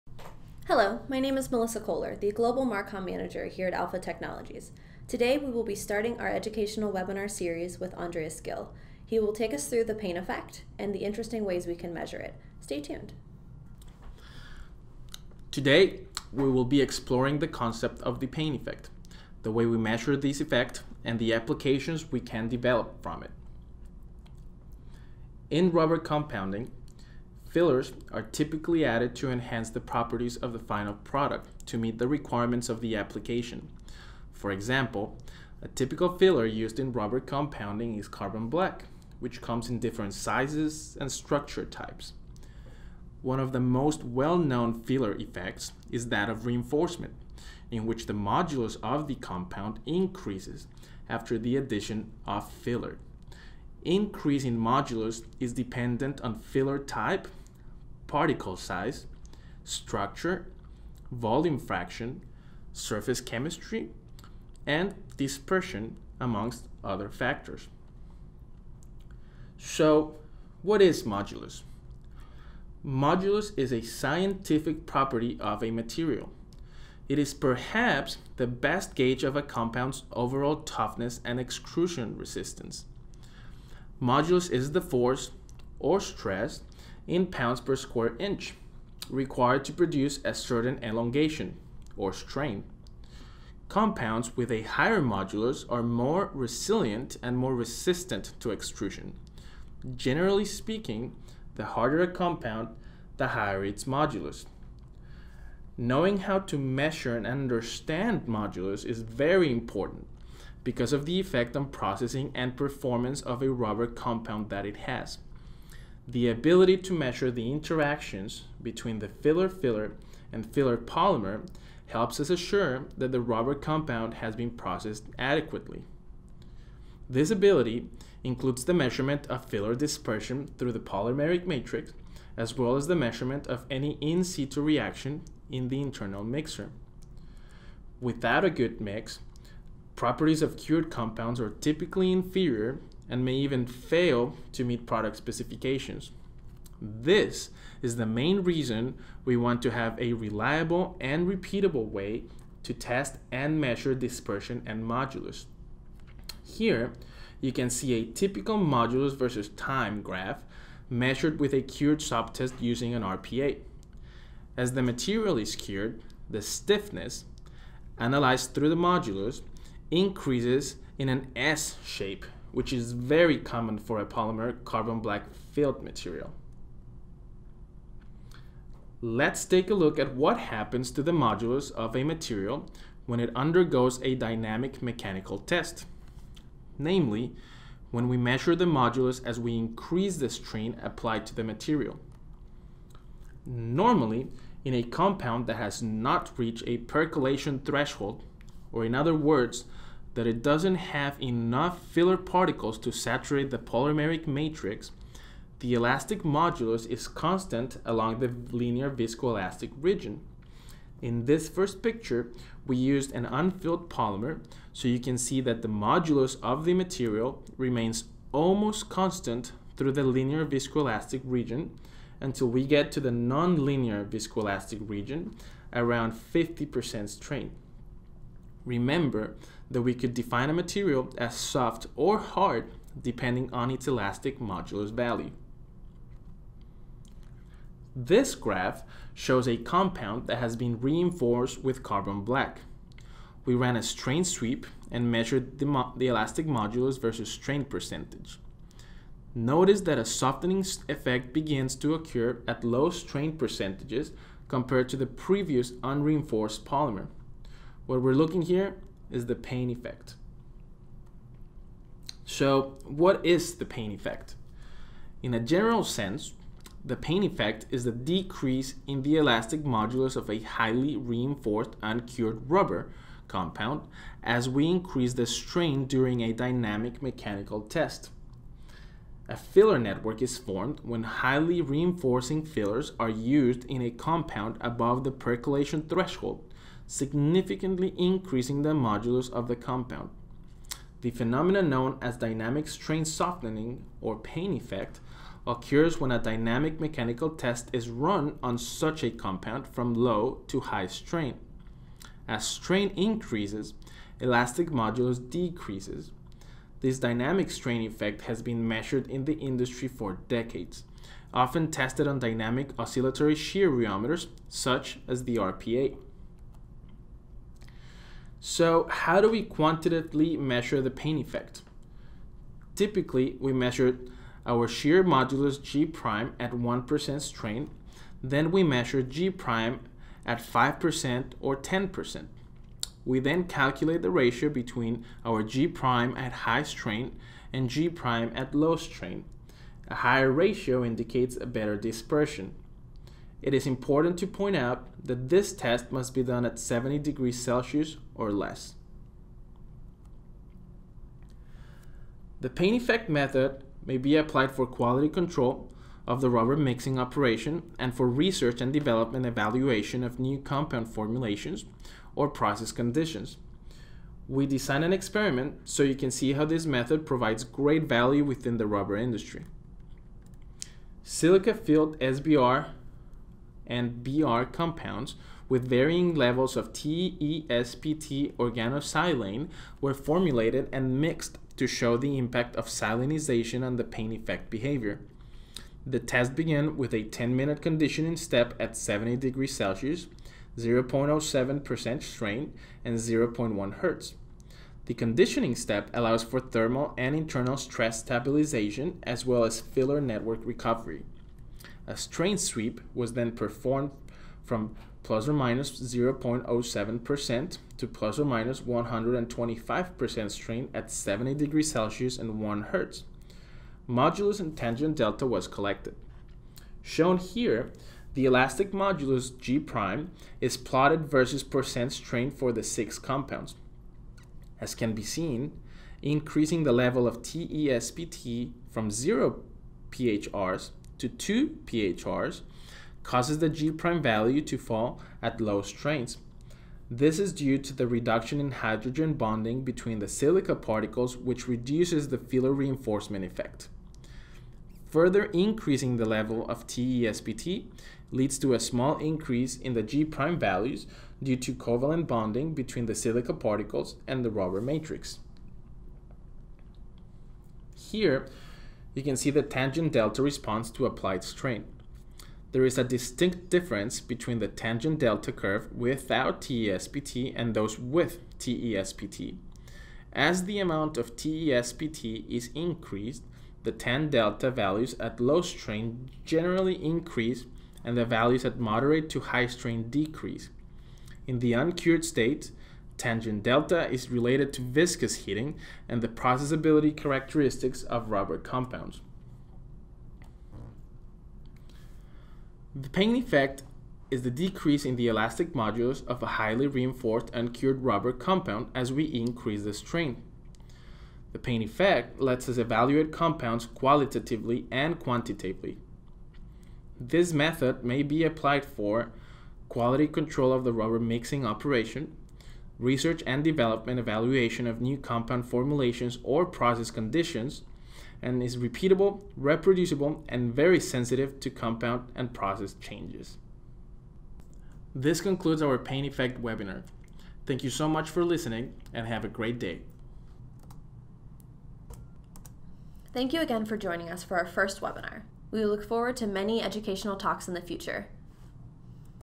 The Payne Effect
Listen as Alpha's Applications Engineer talks through the Payne Effect and why it is important to understand.